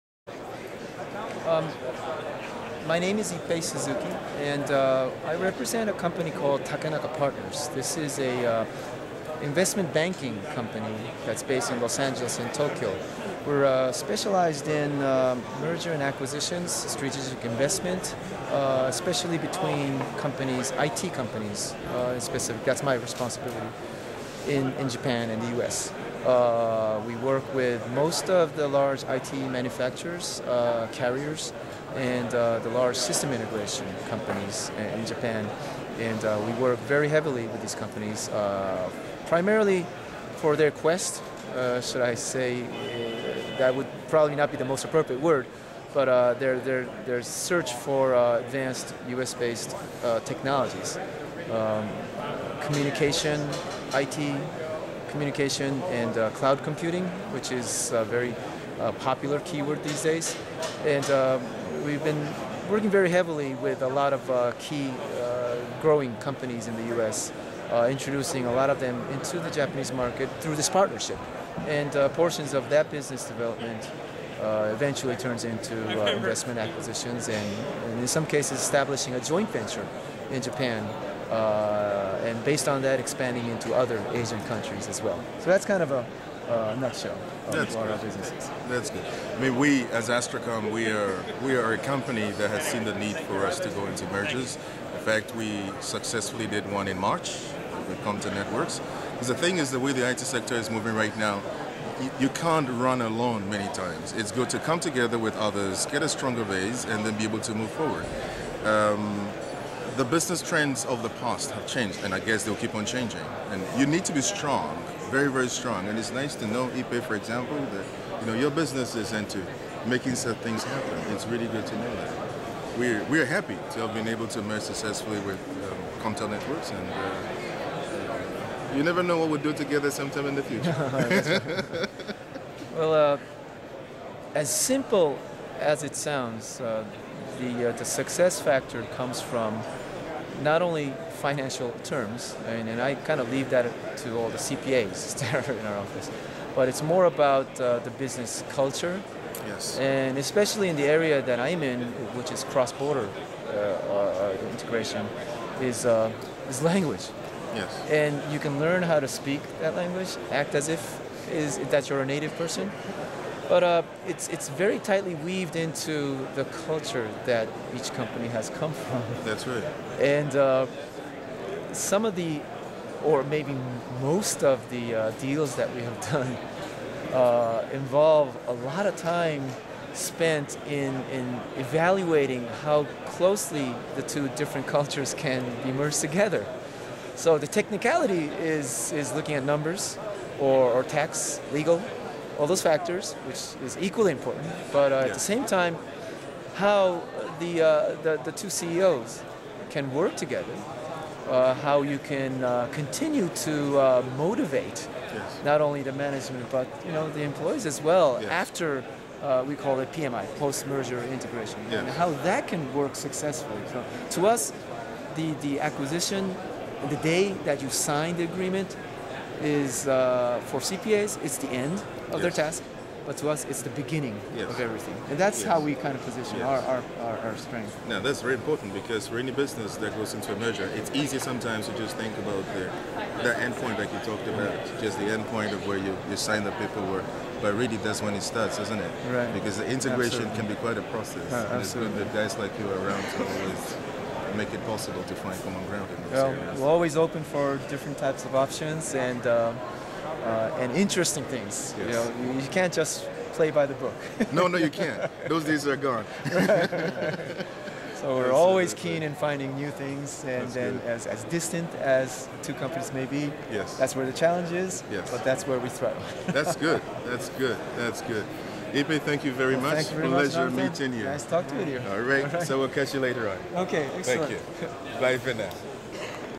Be More "Mergeable, Acquisitionable, and Worth Investing in (at CTIA)
Angel investors. Mergers. Acquisitions. Buyouts. Investments. I met Ippei first at ITEXPO West 2010 in L.A. and then I saw him again at CTIA the same week in San Francisco.

After listening to Jonathan Sowah of AstraQom and Ippei Suzuki of Takenaka Partners discuss these topics, the conclusion is that a lot of time is spent in evaluating how closely can the two cultures of the potential investor/investee or merger or acquisition get along for a successful future. Takenaka Partners specializes in connecting Japanese investors with USA companies.

who were a media partner for CTIA Enterprise and Applications

Notice at the end of the video the booth stand. At the bottom, it says, "Another successful member of DIDXchange." This is common practice among the carriers, operators, service providers, voip companies members of DIDX at conferences with their signage. It only makes my company want to make DIDX better than ever for the wholesale buyers and vendors on DIDXchange.

Suzanne Bowen is the video recorder.